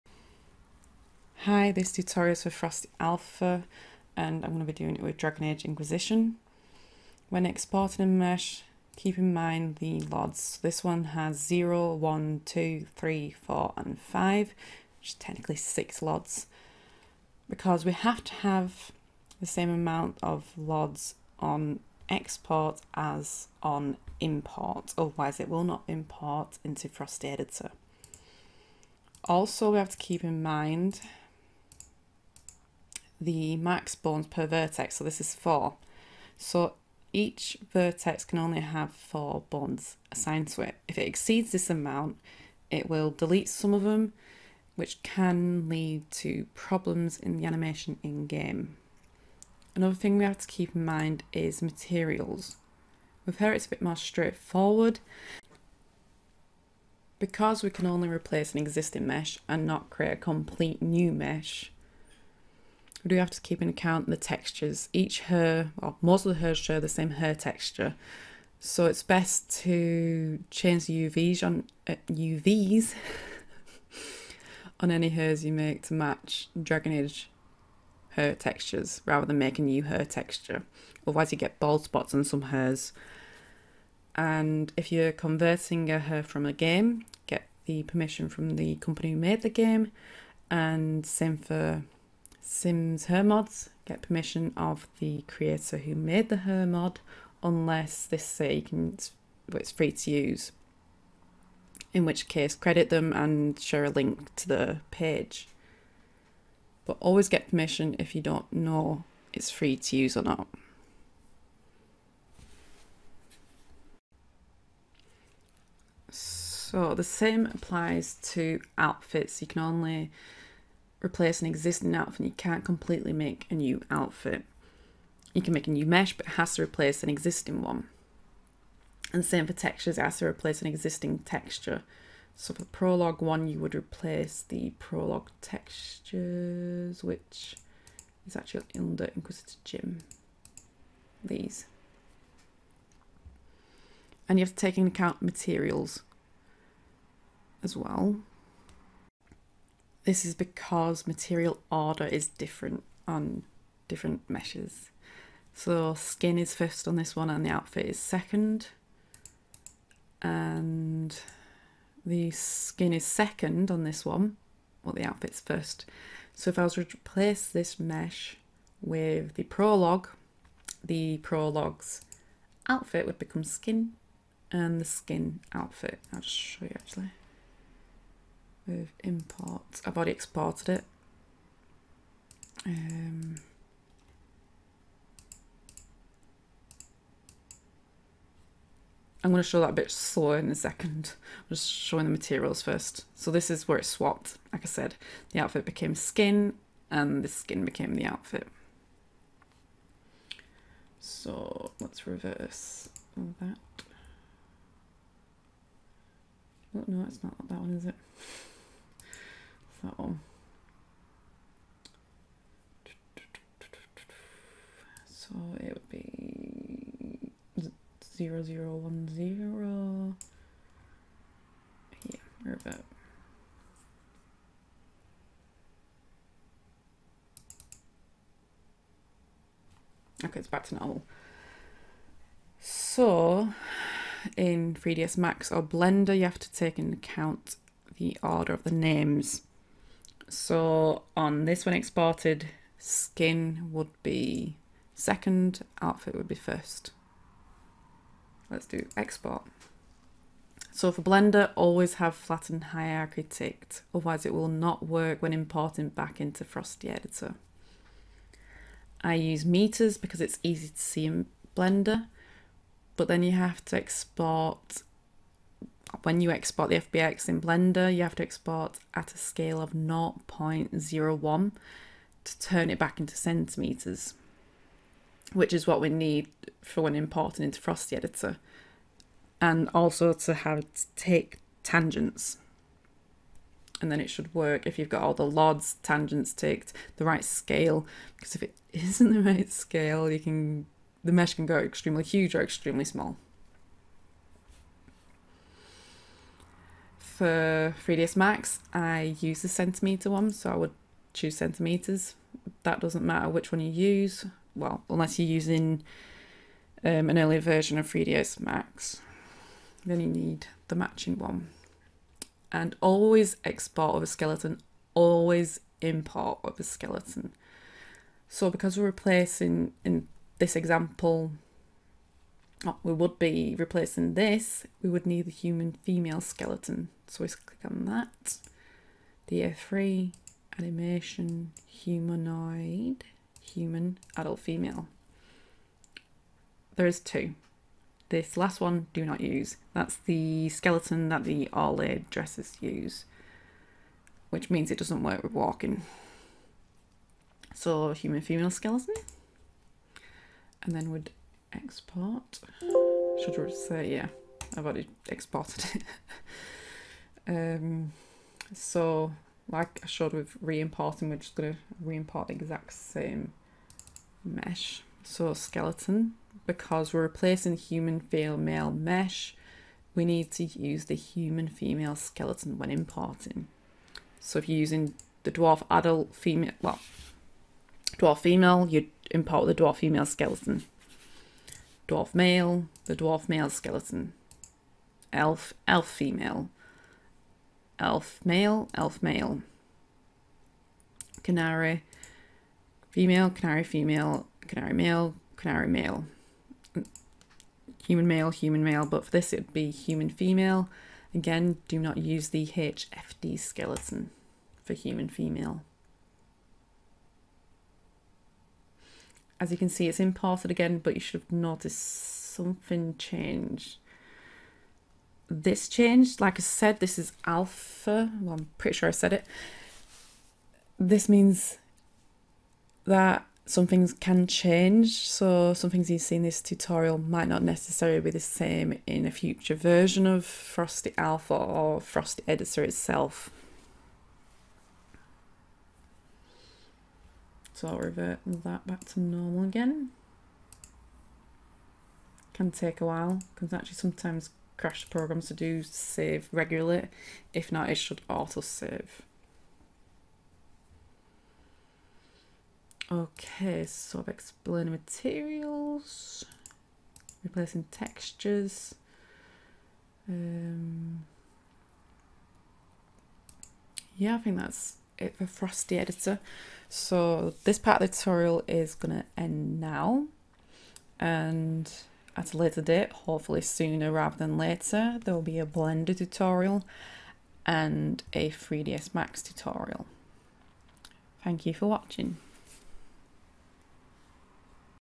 frosty editor tutorial - exporting meshes for use in blender
This is a tutorial I made for Frosty Alpha Editor. Blender and 3ds max tutorials and then importing an edited mesh will come in those too.